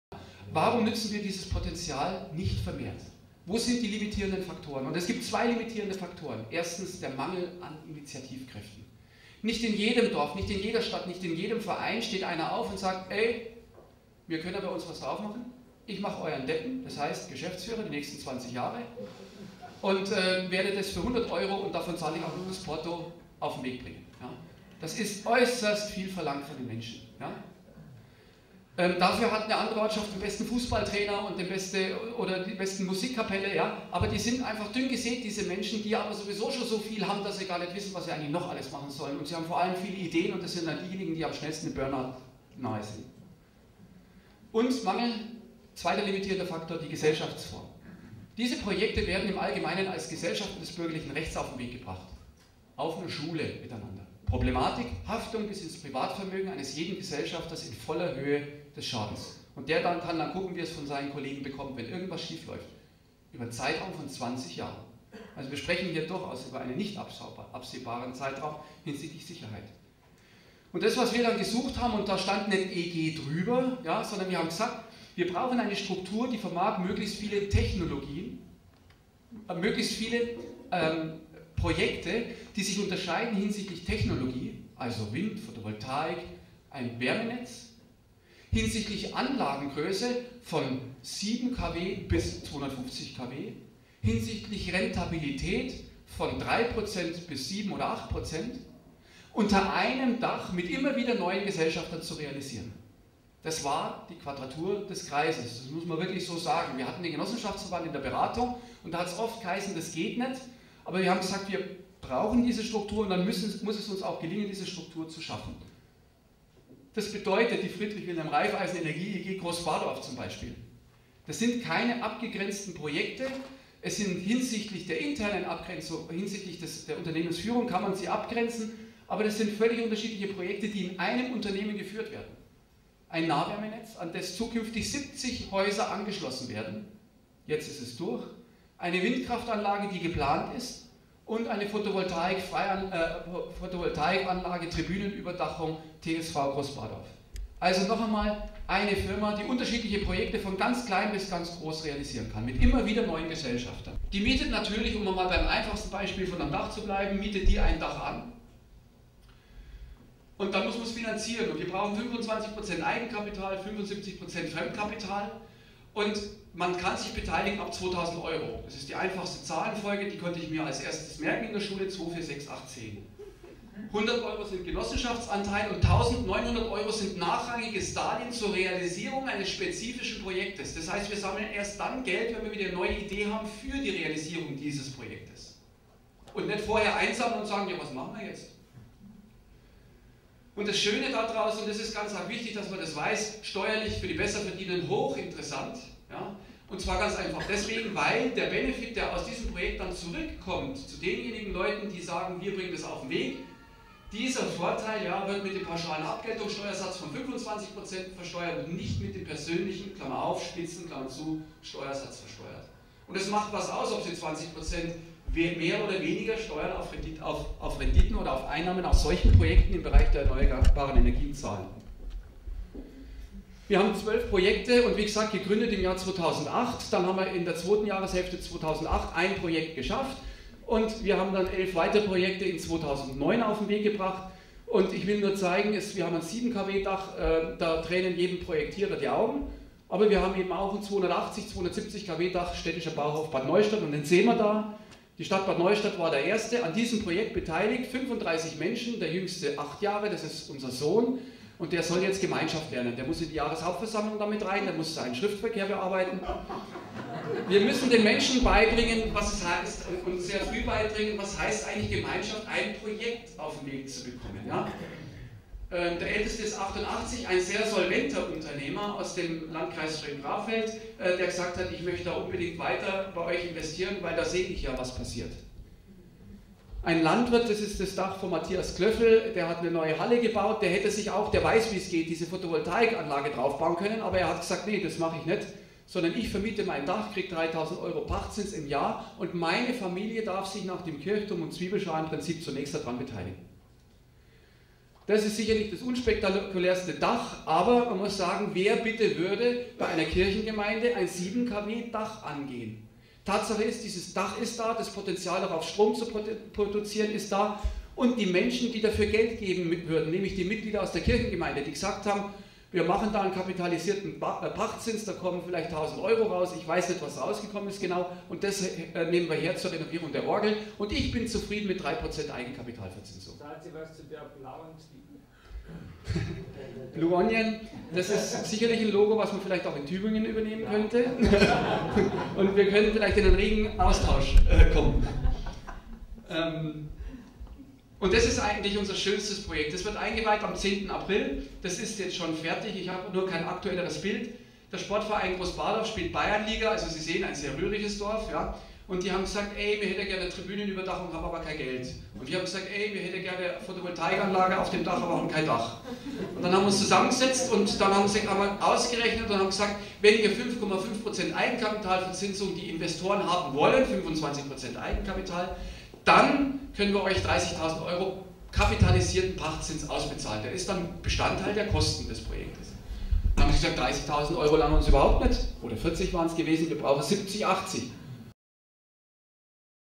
Voller Energie 2010 - Michael Diestel - Teil 4
Vortrag zum Thema "Bürger investieren in Erneuerbare".